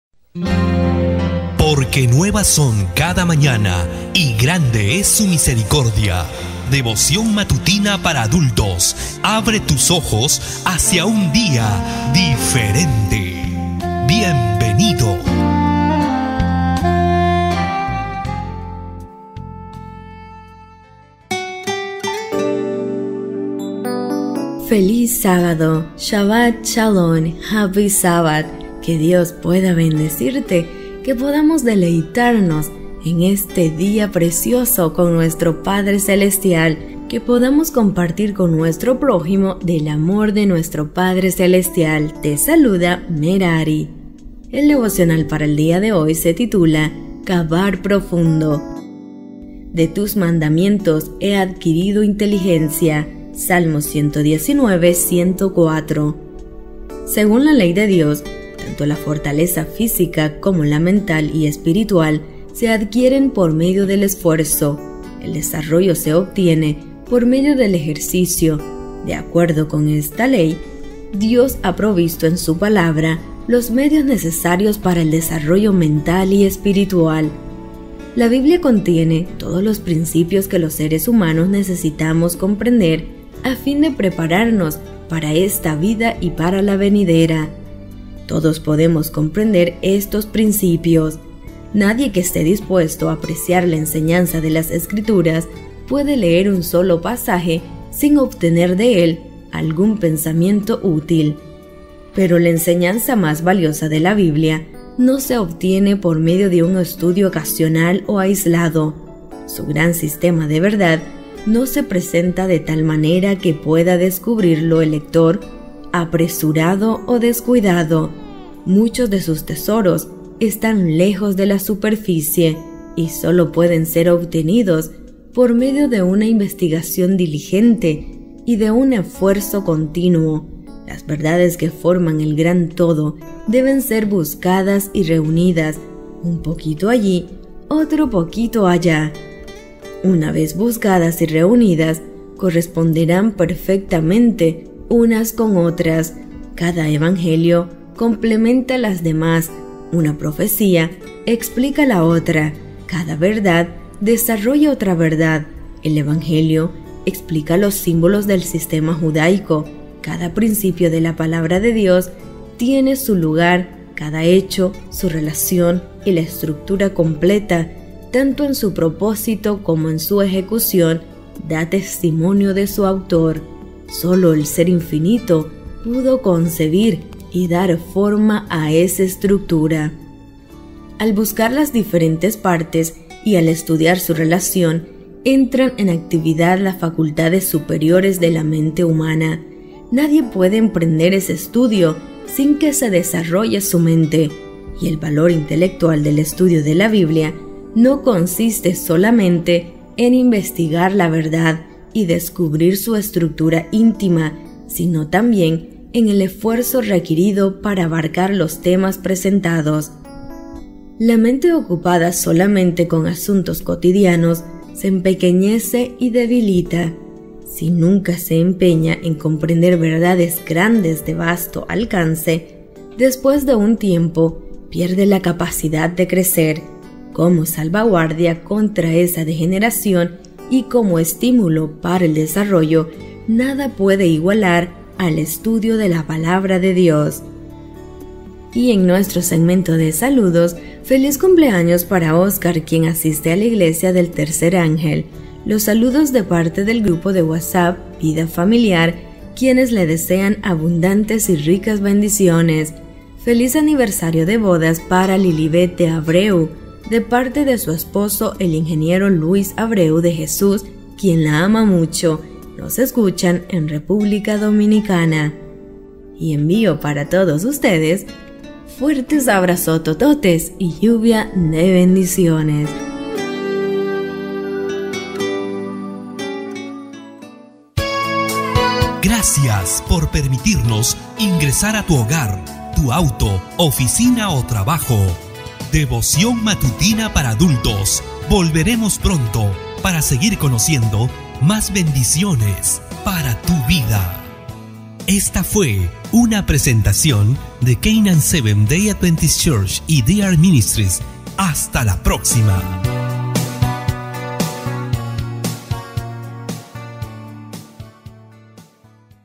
Devoción Matutina Para Adultos - Sábado 14 de Enero del 2017
CAVAR PROFUNDO

«De tus mandamientos he adquirido inteligencia». Salmo 119: 104

SEGÚN LA LEY DE DIOS, tanto la fortaleza física, como la mental y espiritual se adquieren por medio del esfuerzo. El desarrollo se obtiene por medio del ejercicio. De acuerdo con esta ley, Dios ha provisto en su Palabra los medios necesarios para el desarrollo mental y espiritual.

La Biblia contiene todos los principios que los seres humanos necesitamos comprender, a fin de prepararnos para esta vida y para la venidera. Todos podemos comprender estos principios. Nadie que esté dispuesto a apreciar la enseñanza de las Escrituras puede leer un solo pasaje sin obtener de él algún pensamiento útil. Pero la enseñanza más valiosa de la Biblia no se obtiene por medio de un estudio ocasional o aislado. Su gran sistema de verdad no se presenta de tal manera que pueda descubrirlo el lector apresurado o descuidado. Muchos de sus tesoros están lejos de la superficie, y solo pueden ser obtenidos por medio de una investigación diligente y de un esfuerzo continuo. Las verdades que forman el gran todo deben ser buscadas y reunidas «un poquito allí, otro poquito allá» (Isa. 28: 10, RV60).

Una vez buscadas y reunidas, corresponderán perfectamente unas con otras. Cada Evangelio complementa los demás; una profecía explica la otra; cada verdad desarrolla otra verdad. El evangelio explica los símbolos del sistema judaico. Cada principio de la Palabra de Dios tiene su lugar; cada hecho, su relación. Y la estructura completa, tanto en su propósito como en su ejecución, da testimonio de su Autor. Solo el Ser infinito pudo concebir y dar forma a esa estructura.

Al buscar las diferentes partes y al estudiar su relación, entran en actividad las facultades superiores de la mente humana. Nadie puede emprender ese estudio sin que se desarrolle su mente.

Y el valor intelectual del estudio de la Biblia no consiste solamente en investigar la verdad y descubrir su estructura íntima, sino también en el esfuerzo requerido para abarcar los temas presentados. La mente ocupada solamente con asuntos cotidianos se empequeñece y debilita. Si nunca se empeña en comprender verdades grandes y de vasto alcance, después de un tiempo pierde la capacidad de crecer. Como salvaguardia contra esa degeneración, y como estímulo para el desarrollo, nada puede igualar al estudio de la Palabra de Dios.— La educación, cap. 13, pp. 111-112.

De vuelta al hogar
Por: Ellen G. White